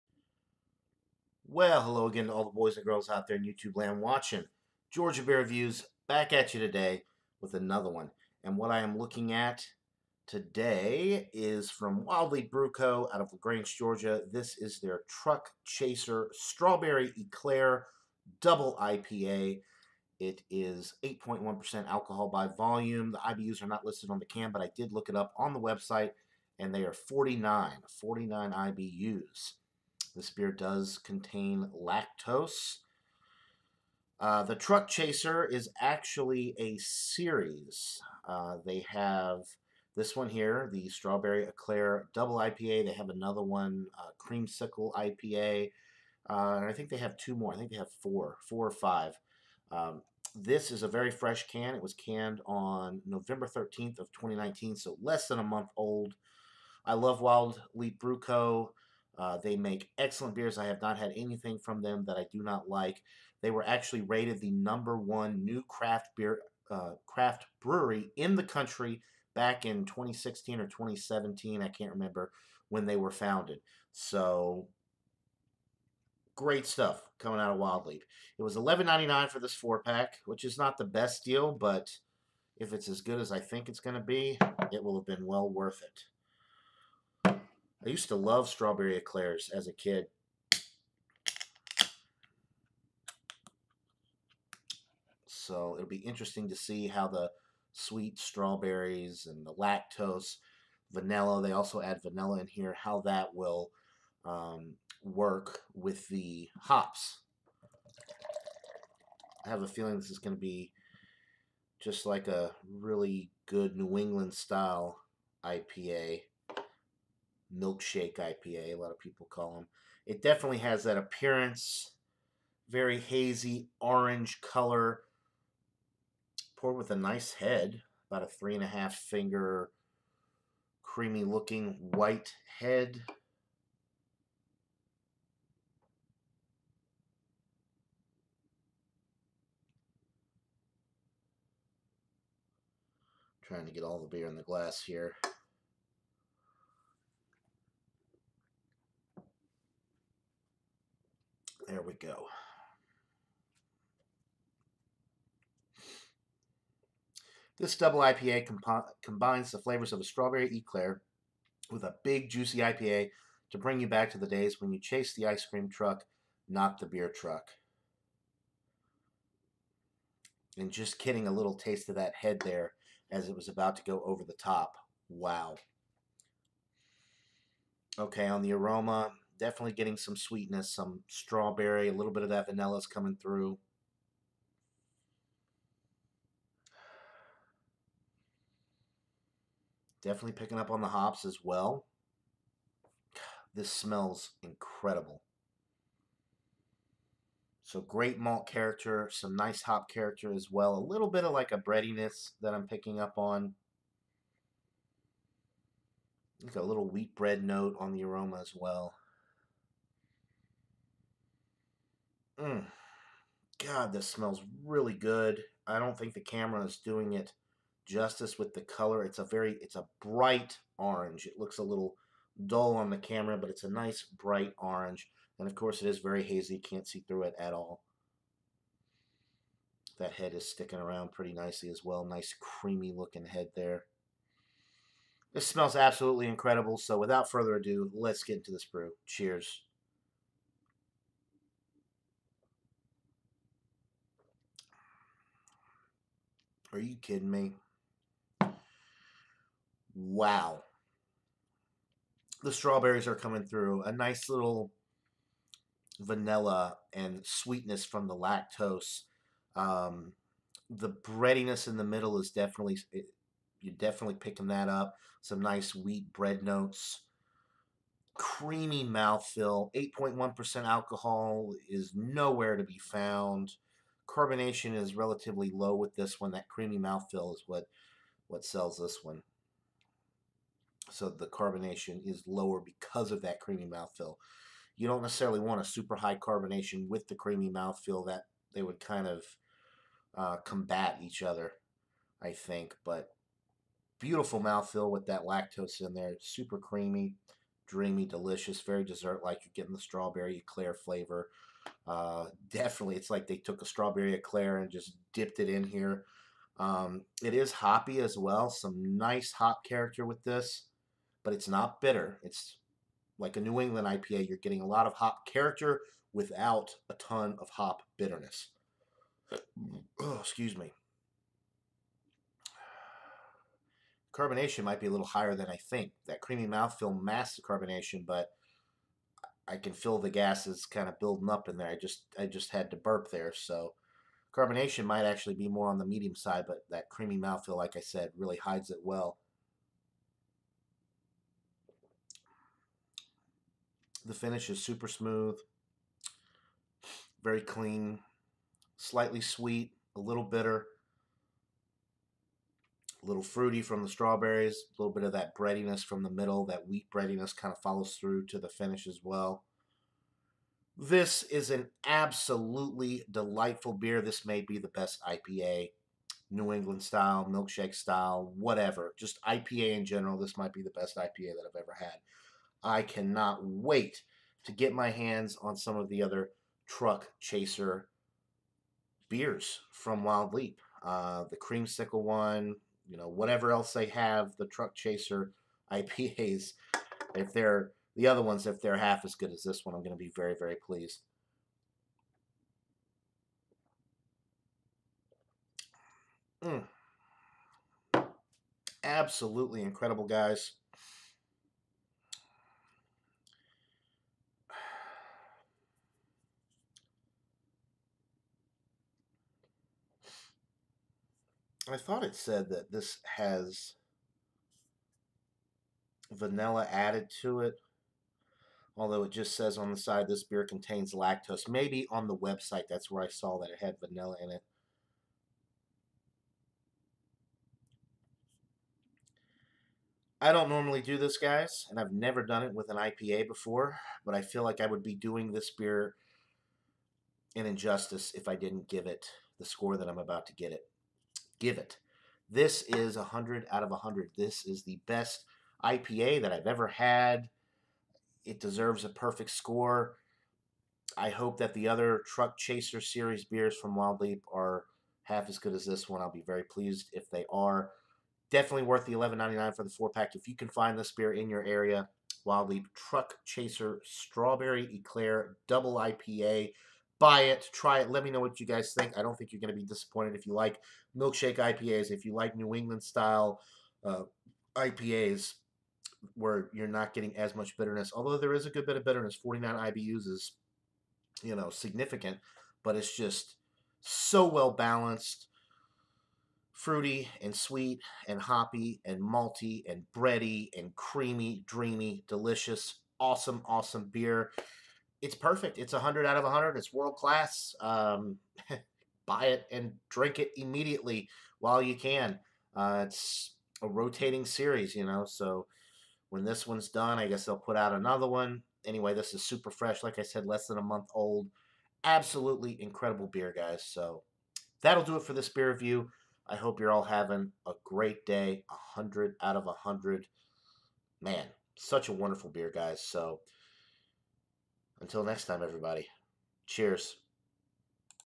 Georgia Beer Reviews: Wild Leap Truck Chaser Strawberry Eclair Double IPA
8.1% alcohol. 49 IBUs. Brewed and canned by Wild Leap Brew Co. in LaGrange, GA.